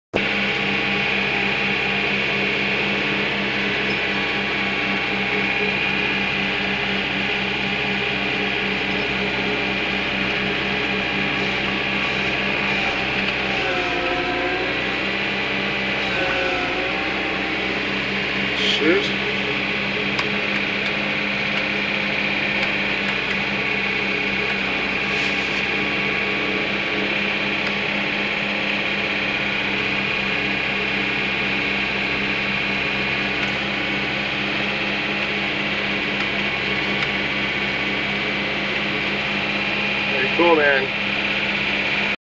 loader mounted blower
From the cab of a John Deere tractor, this hydrualic drive, 2 stage; loader mounted snow blower is sure to impress!